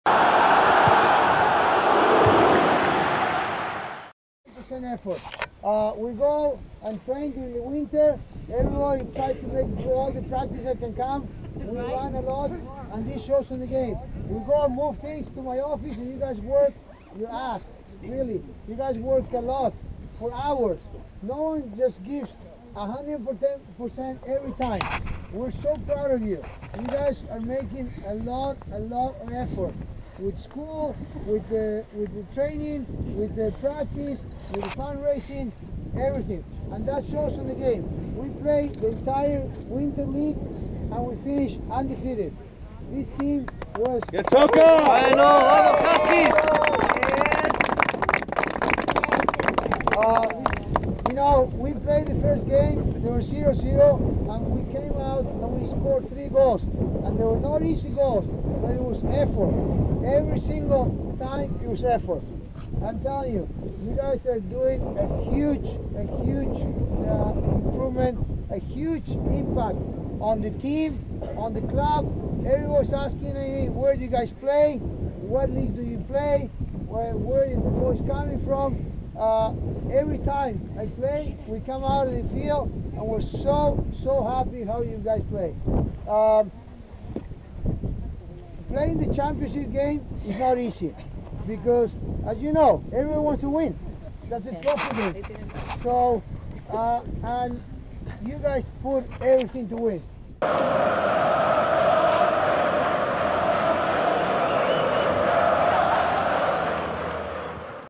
TOCATV: Winter Outdoor League (Championship Game - Congratulations Words)
Sunday, February 26, 11:45 AM
U15 Blue Metros vs U16 Boys HUSKIES
MDSoccerPlex #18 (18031 Central Park Circle, Boyds, MD 20841)